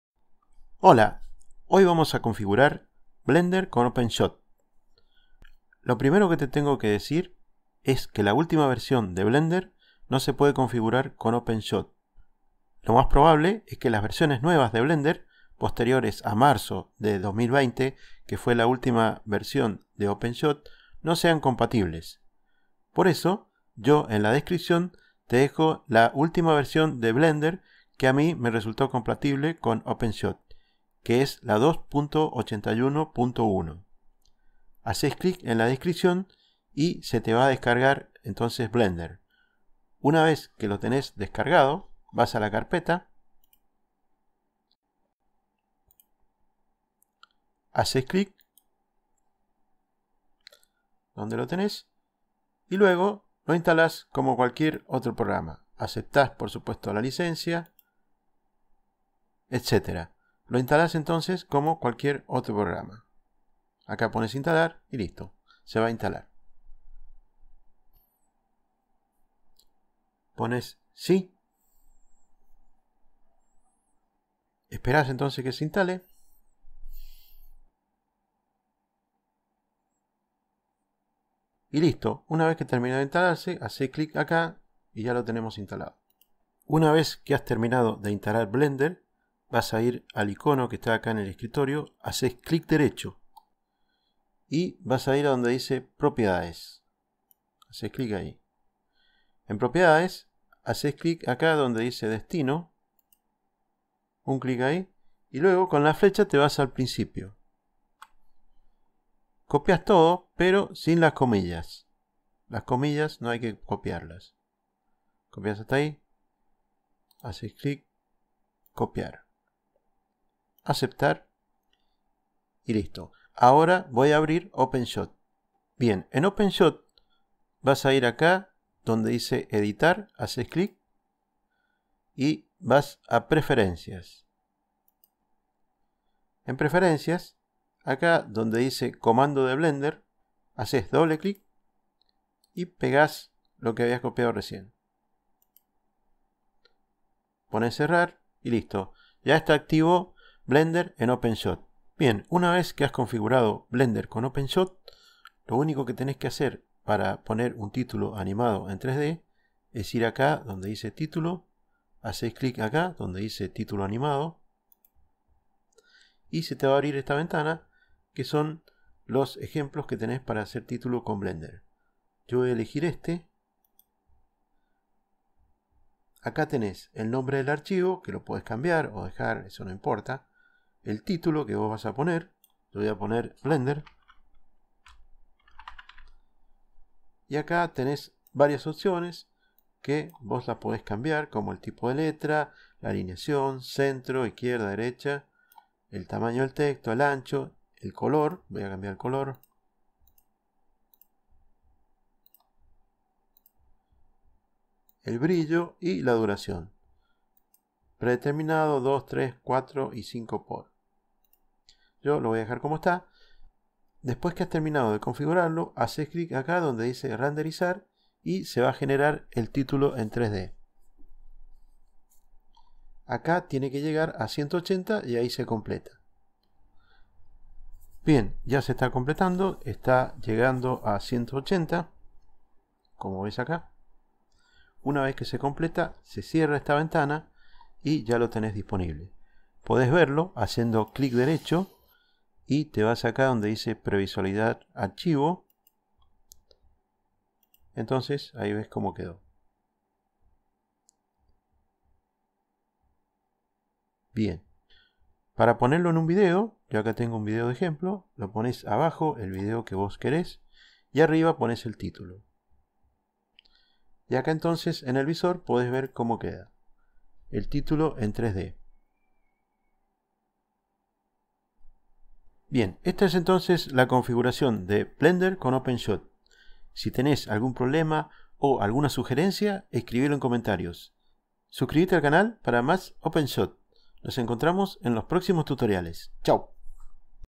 Como configurar Blender con Openshot / Como hacer un título en 3 D
Tutorial paso a paso para instalar la versión de Blender compatible con Openshot y como configurarlo. Además como hacer un título en 3D.
Link de descarga Blender 2.81a
👍Si te sirvio darme un like para saberlo ¡👋Gracias!